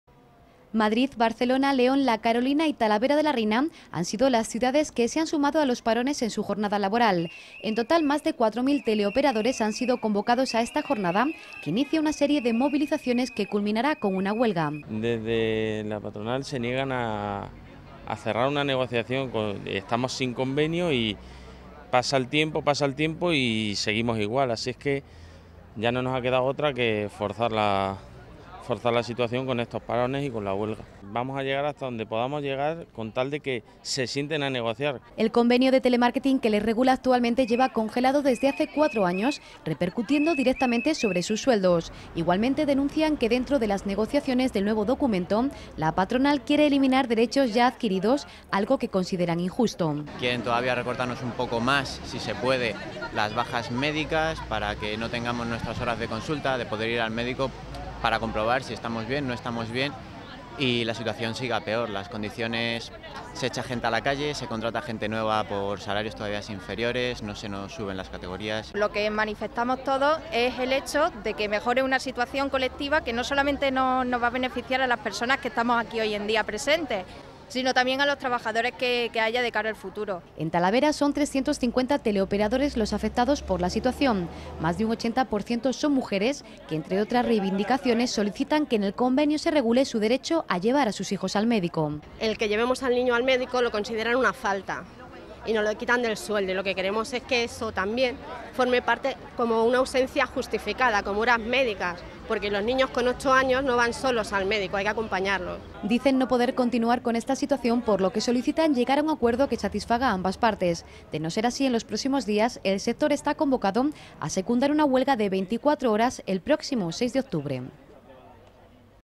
LOS TRABAJADORES DE DIGITEX IRÁN A LA HUELGA EL 6 DE OCTUBRE
Tele operadores de Talavera de la Reina, la Carolina, Madrid, Barcelona y León cuelgan durante unas horas sus cascos para empezar paros parciales de dos horas en su jornada laboral durante todos los  jueves, hasta que se celebre la huelga convocada para el 6 de octubre. Los trabajadores de Digitex salen a la calle en señal de protesta para pedir que se tomen cartas en el asunto y se elabore un nuevo convenio.